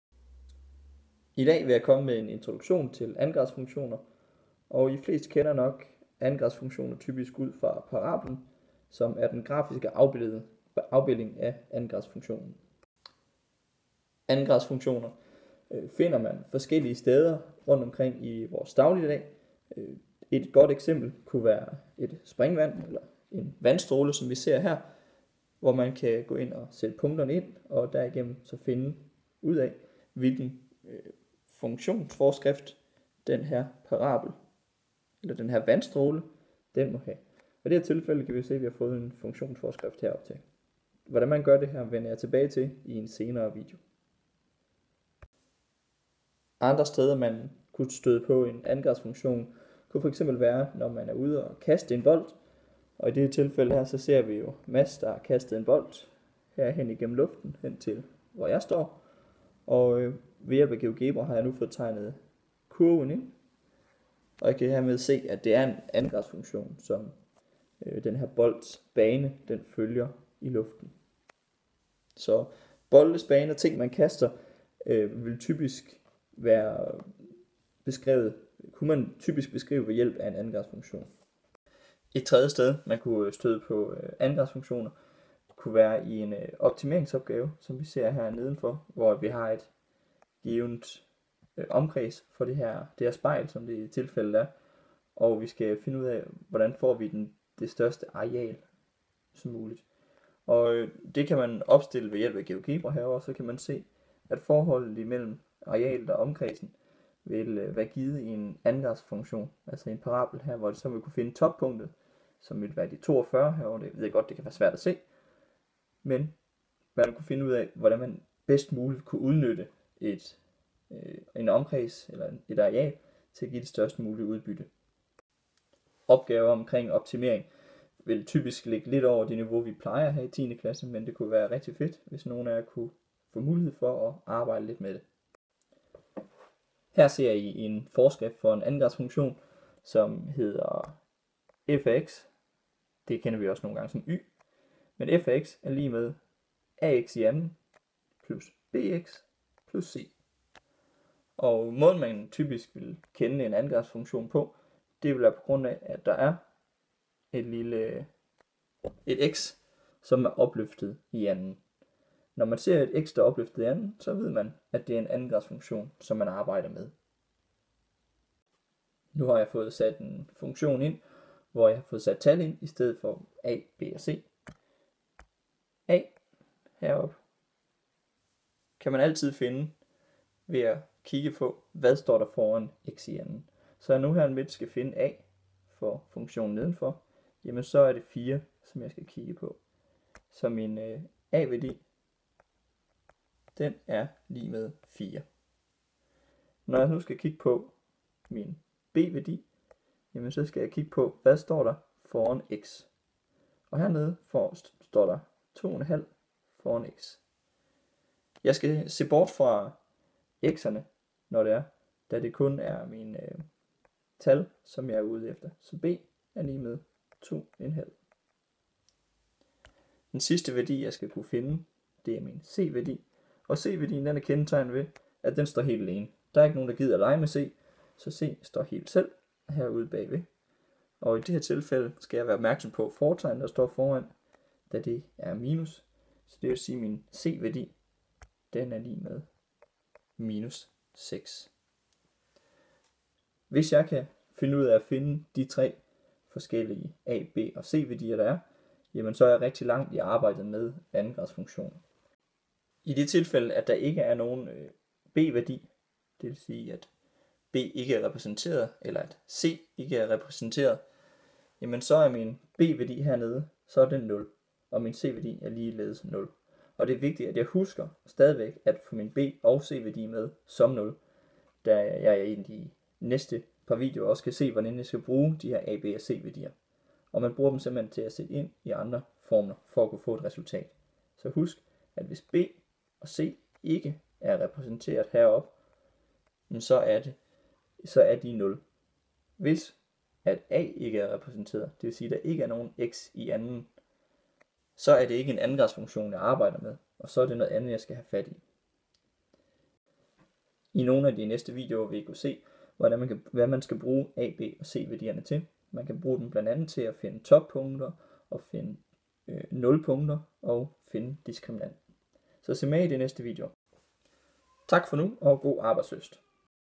Introduktion til andengradsfunktioner
En introduktion til hvor du finder andengradsfunktioner og hvordan de er bygget op.